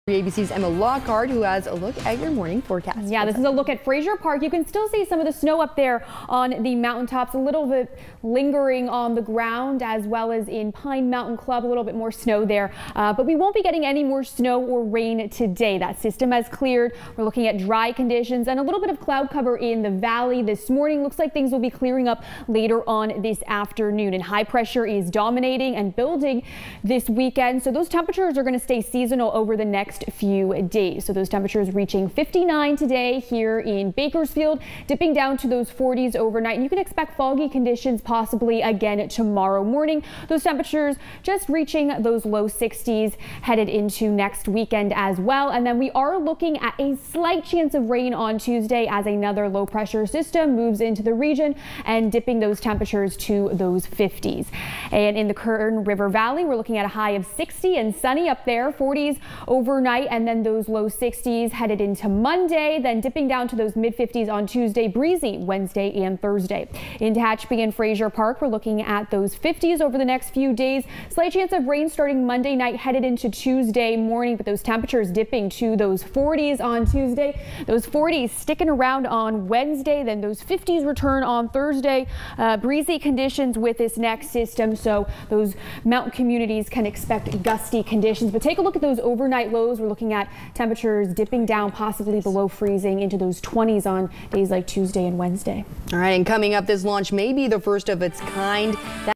Dry and stable this weekend
Temperatures will stay seasonal this weekend. Dry conditions and light wind speeds are expected. Slight chance of rain starting Monday night until Tuesday morning.